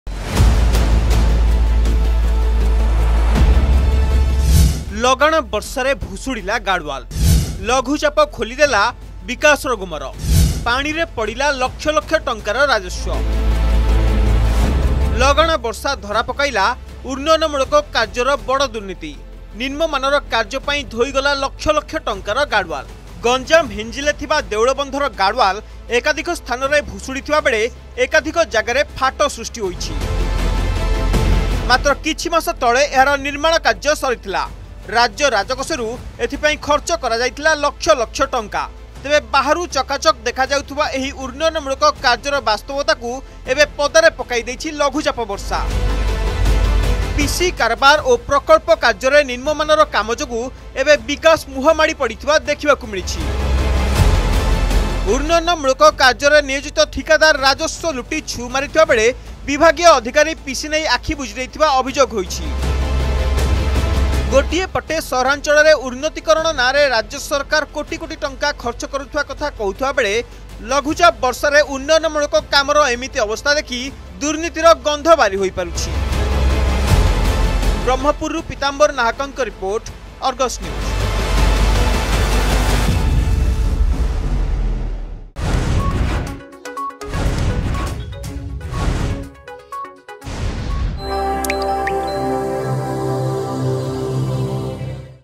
ଲଗାଣ ବର୍ଷାରେ ଭୁଶୁଡିଲା ଗାର୍ଡଓ୍ବାଲ କାନ୍ଥ
ଲଘୁଚାପ ଖୋଲିଦେଲା ବିକାଶର ଗୁମର
ପାଣିରେ ପଡିଲା ଲକ୍ଷ ଲକ୍ଷ ଟଙ୍କାର ରାଜସ୍ବ
ଲଗାତାର ବର୍ଷା ଧରା ପକାଇଲା ଉନ୍ନୟନମୂଳକ କାର୍ଯ୍ୟର ବଡ଼ ଦୁର୍ନୀତି । ନିମ୍ନମାନର କାର୍ଯ୍ୟ ପାଇଁ ଧୋଇଗଲା ଲକ୍ଷ ଲକ୍ଷ ଟଙ୍କାର ଗାର୍ଡୱାଲ । ଗଞ୍ଜାମ ହିଂଜିଳିରେ ଥିବା ଦେଉଳବନ୍ଧ ର ଗାର୍ଡୱାଲ ଏକାଧିକ ସ୍ଥାନରେ ଭୁଶୁଡ଼ିଥିବା ବେଳେ ଏକାଧିକ ଜାଗାରେ ଫାଟ ସୃଷ୍ଟି ହୋଇଛି ।